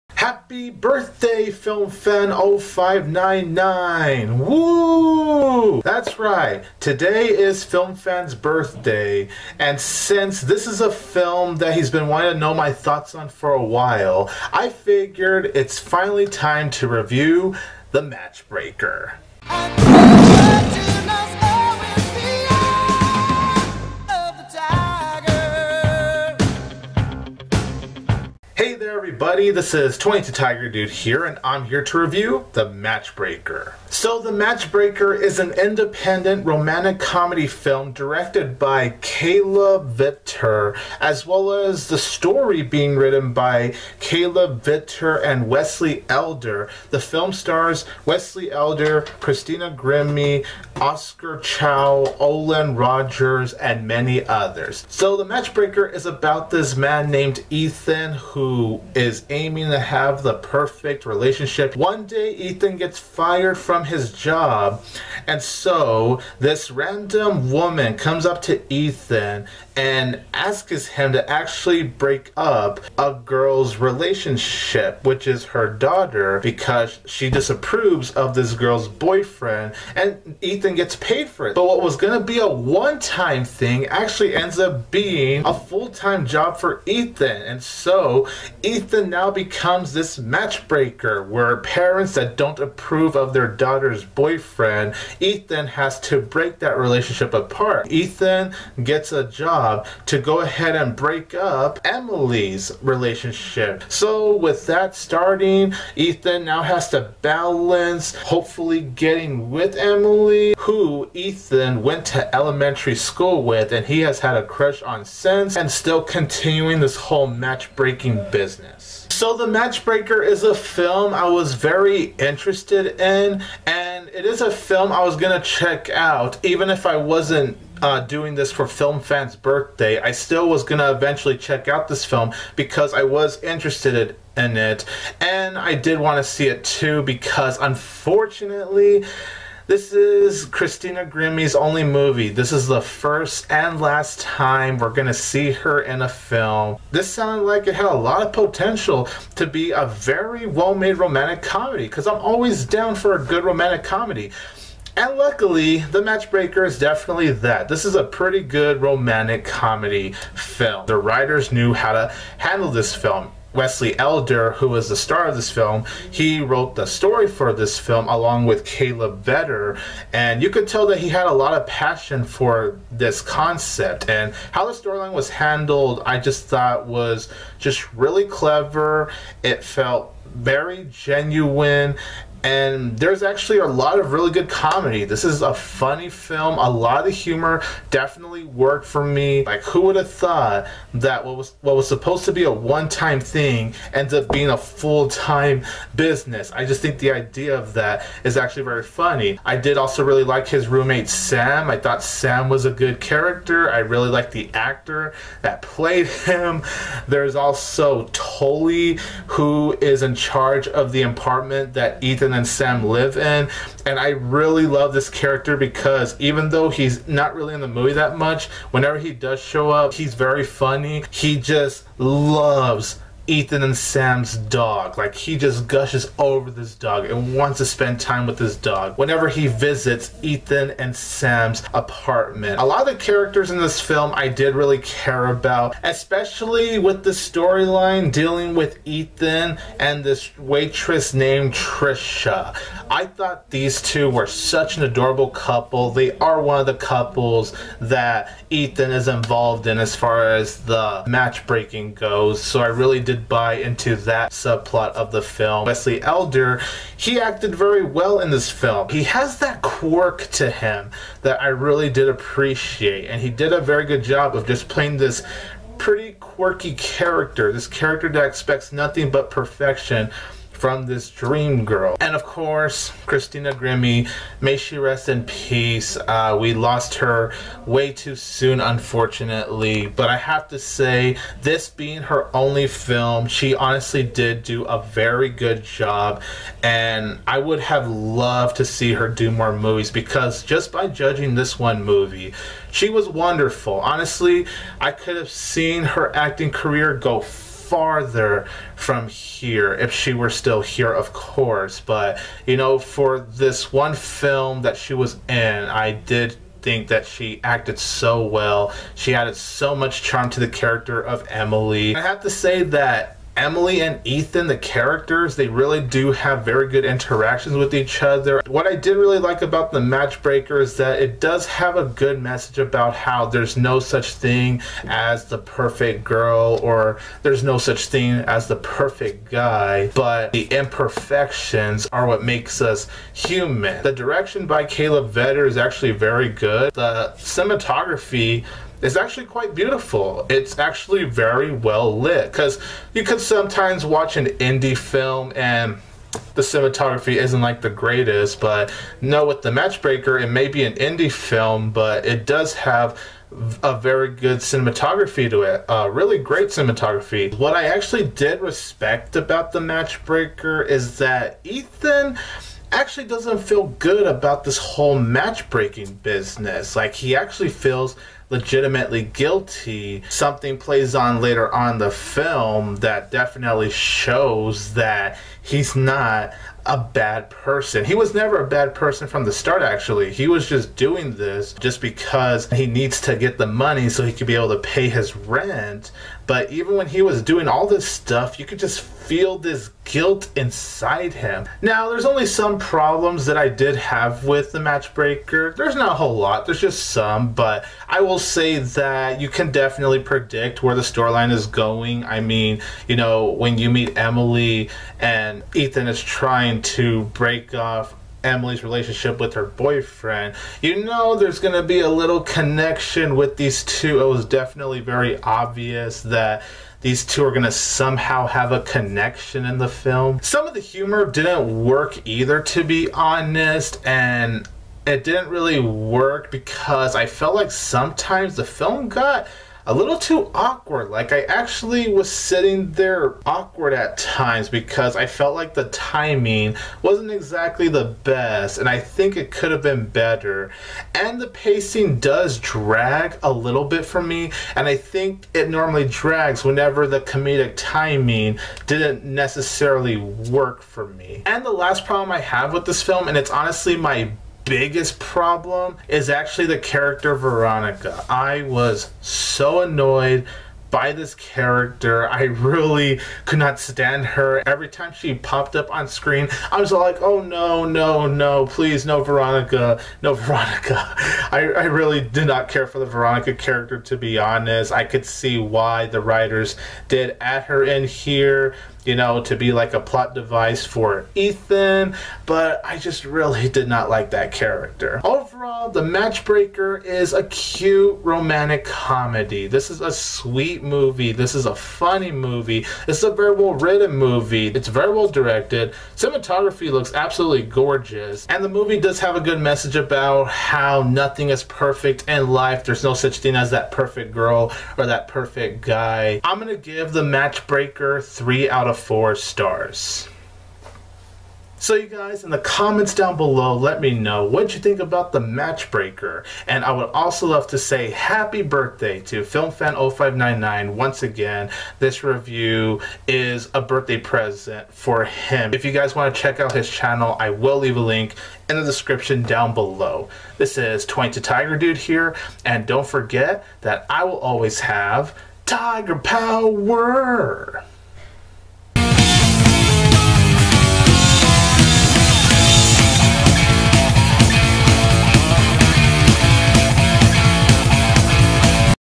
The Matchbreaker movie review
22TigerDude reviews the romance comedy film written by Wesley Elder and Caleb Vetter. Directed by Caleb Vetter. Starring Wesley Elder and Christina Grimmie. What are his thoughts on this film? find out here.

FilmFan's Birthday - May 13th